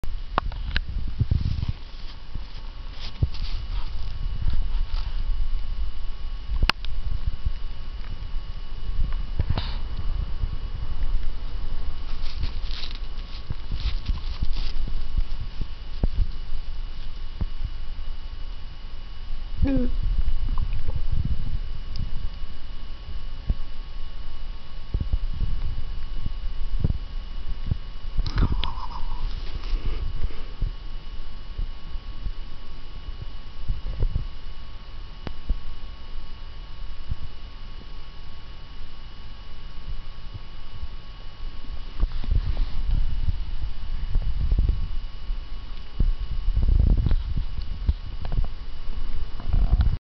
extracting a sea urchin spike
Extracting a sea urchin spike that was on my foot for 3 months.
It went in under the toe and came out on top.